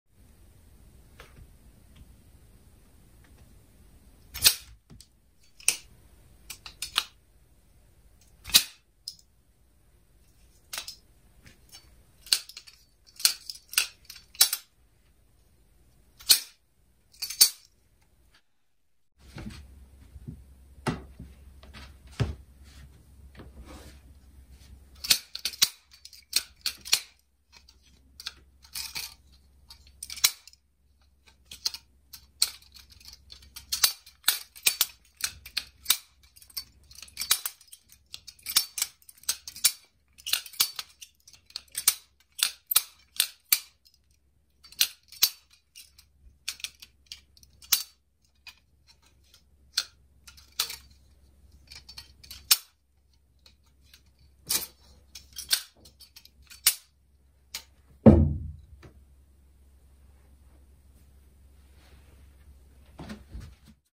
ノコギリで遊んでみた！
実用性皆無の謎スキルを修得してもうた
※安全性を考慮して総歯もげに加工しています